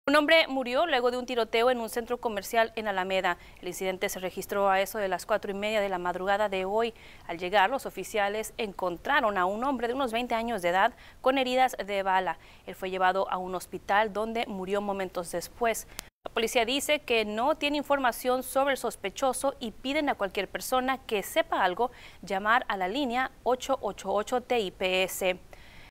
Muere hombre tras tiroteo
Un hombre murió luego de un tiroteo en un centro comercial en alameda.